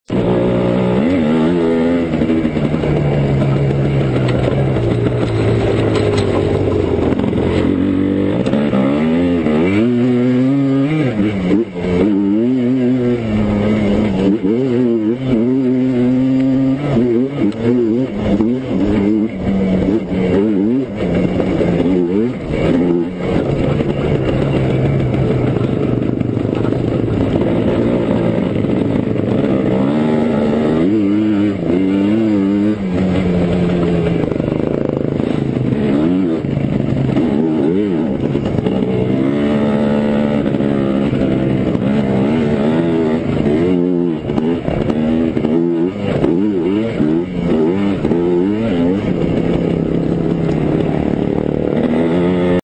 4dirt.sc Can't see the whoops, but they're THERE!
When you're staring at the gas tank, it's because I'm leaned back as far as I can and trying to get on the gas to soak em up!  We weren't really going that fast though so only on the gas a little.  Extended A arms make this SOOO much easier!  Even with stock shocks